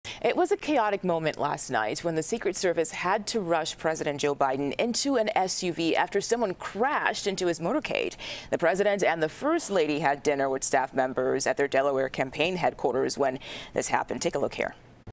Car crashes into Biden's motorcade in Delaware
The president and first lady just finished having dinner with staff members at their Delaware campaign headquarters.